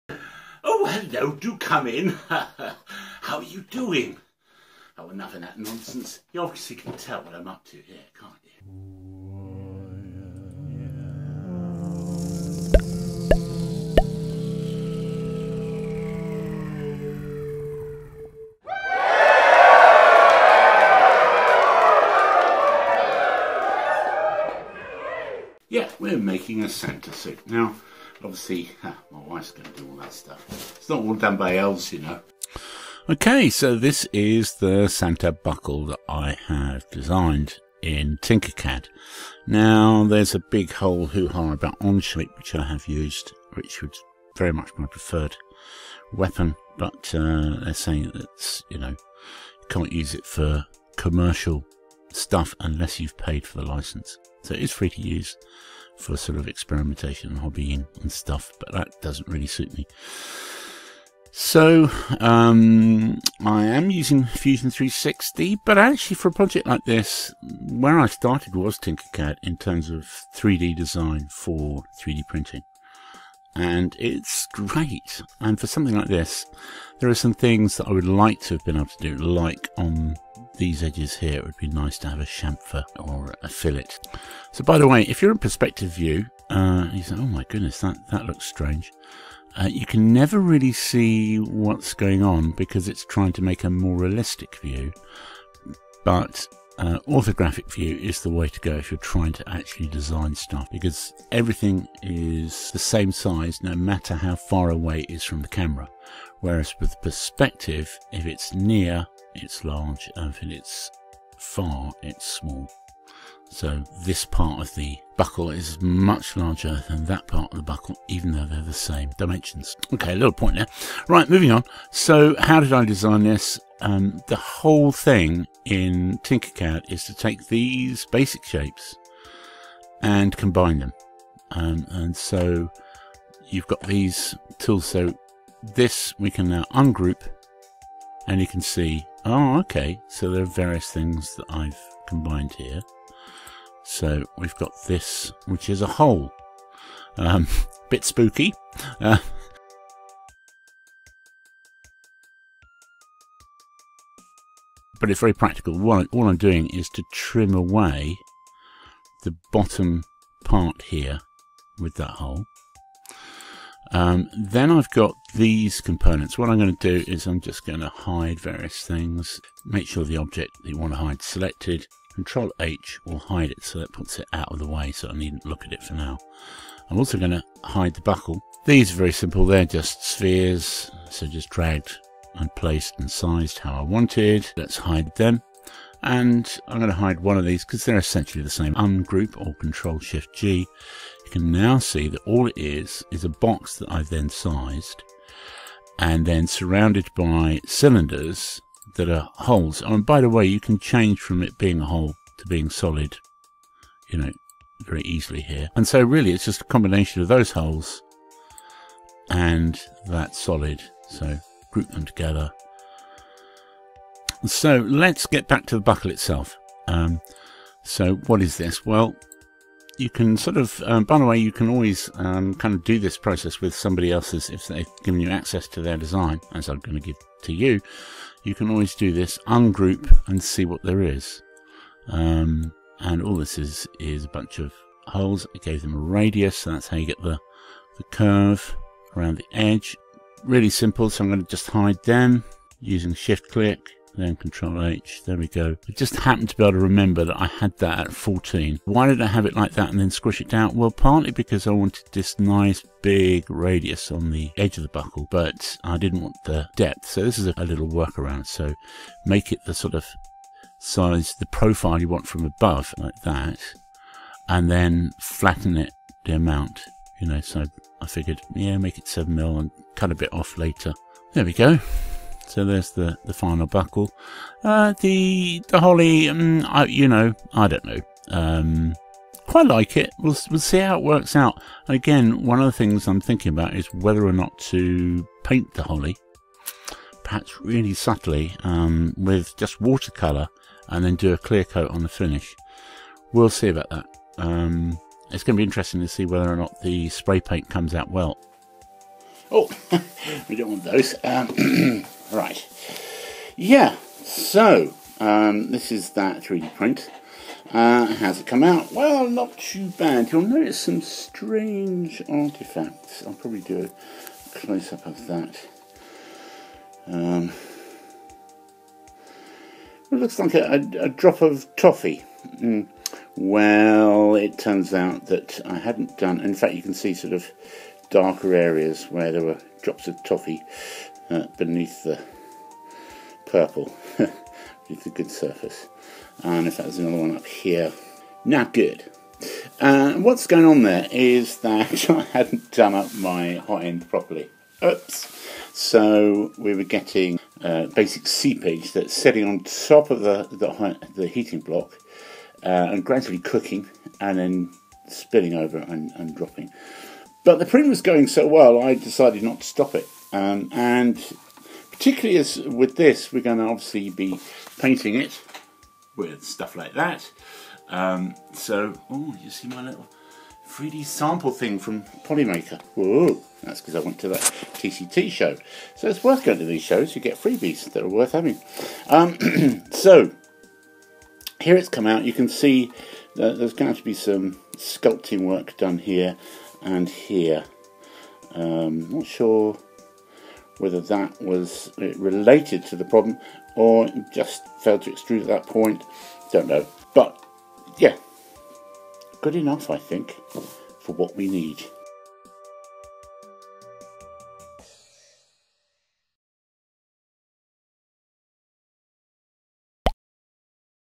Make a Father Christmas (AKA Santa) Belt Part 1 (Making + Tutorial + 3D) WARNING spoilers for kids!
A lot of people already know I'm the real Father Christmas (AKA Santa) - but please don't spoil it for the kids. The elves do a lot of work, but actually I'm making my own belt with 3D printing and other craft techniques - come along for the ride! :)

FlyingHinges Flying Hinges nielsencs captainflyingcarpet